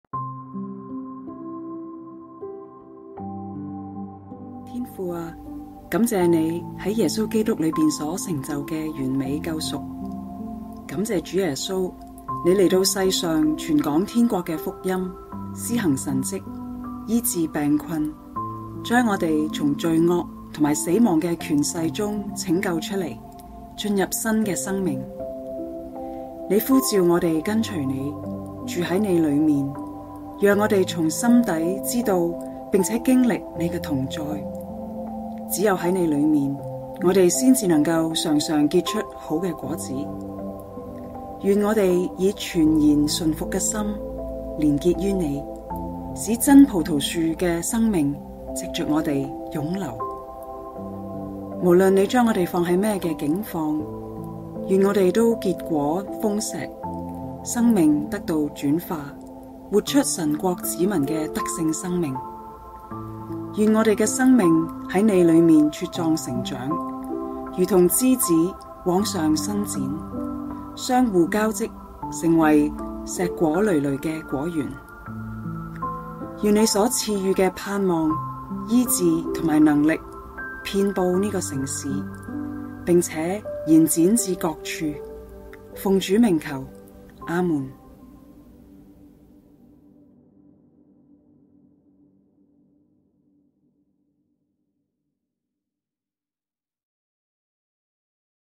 「我是真葡萄樹」禱文｜ 耶穌的七個「我是」｜「一源2022」聆聽聖經運動
🍃稍息。靜聽🏝️
⏳邀請你騰出一分鐘，以主的名字向祂呼求🙋🏼‍♂️🙋🏽，全心、全身、全意向主敞開，沉浸仰望祂話語的瑰麗與奇妙。✨